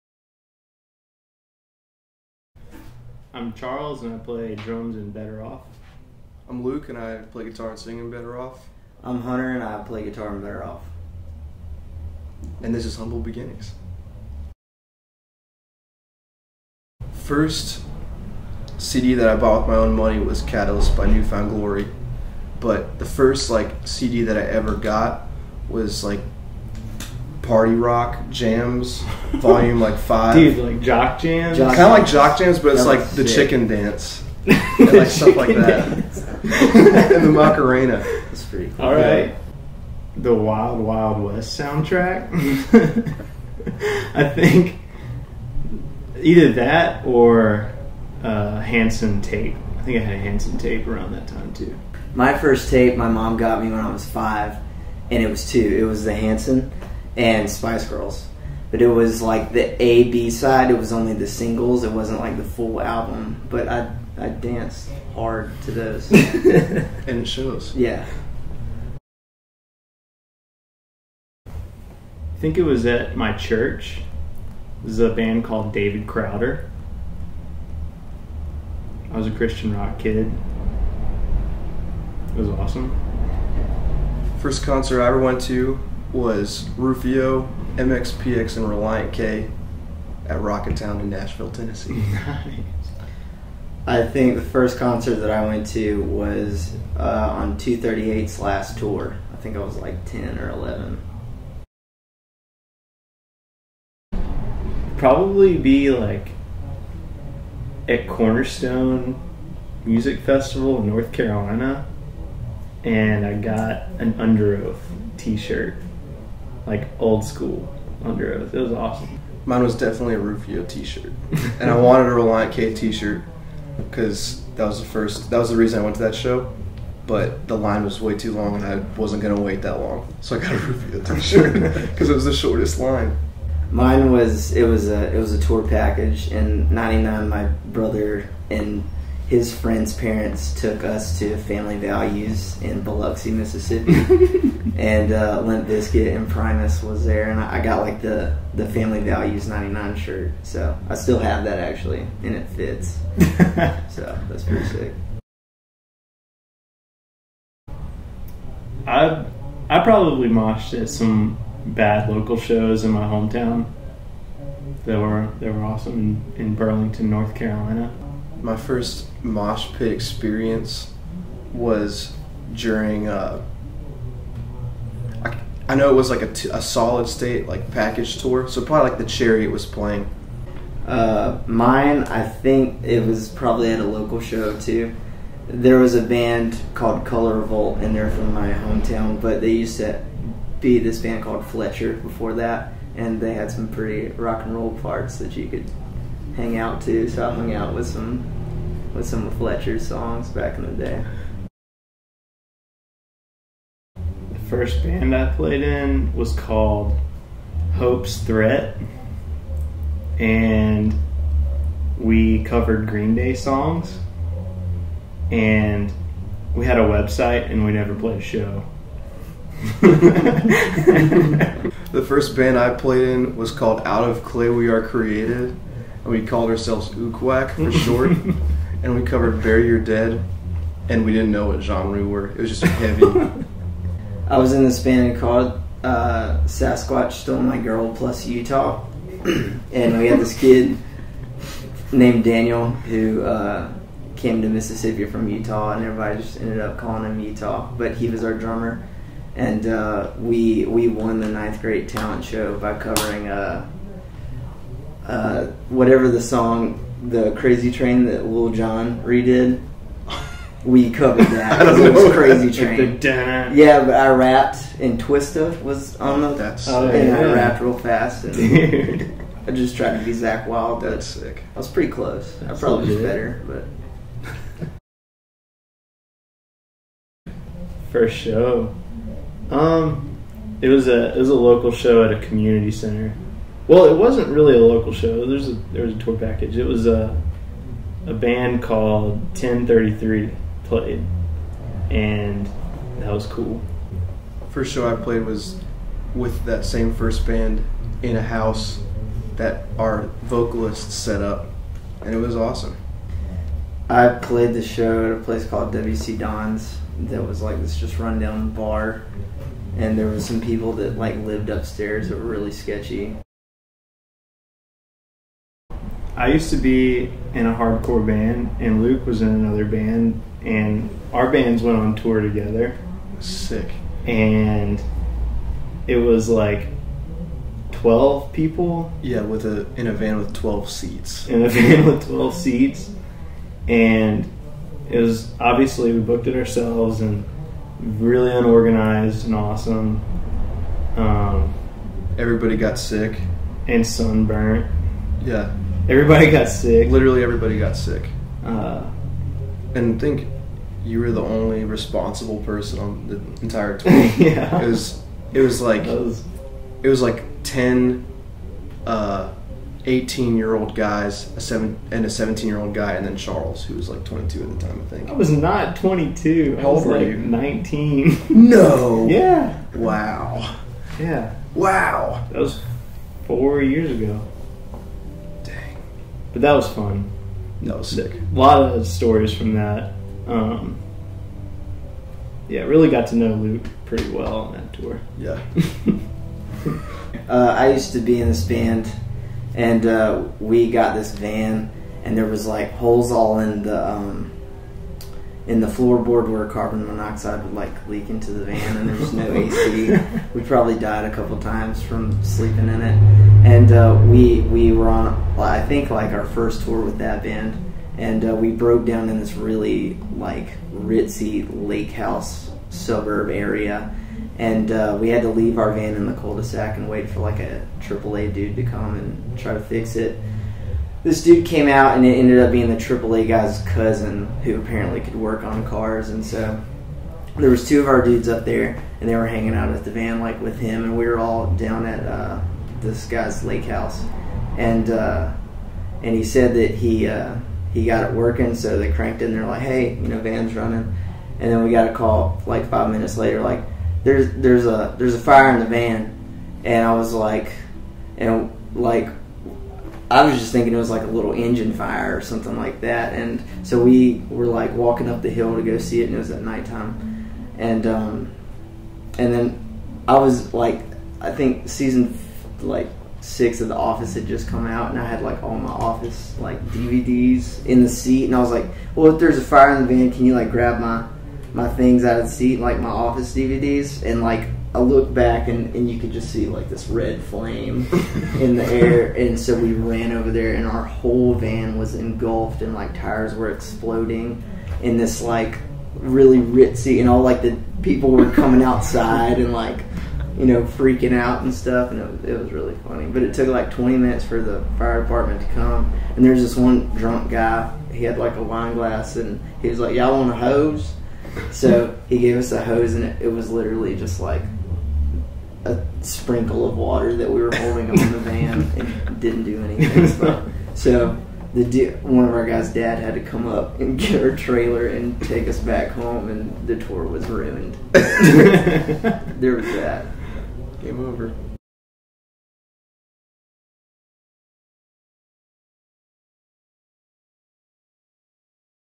No Sleep Records' Humble Beginnings with Better Off
We had the pleasure of interviewing Better Off at the Blood & Ink Office in Richmond, VA for our Humble Beginnings series. Watch as we find out all about their firsts!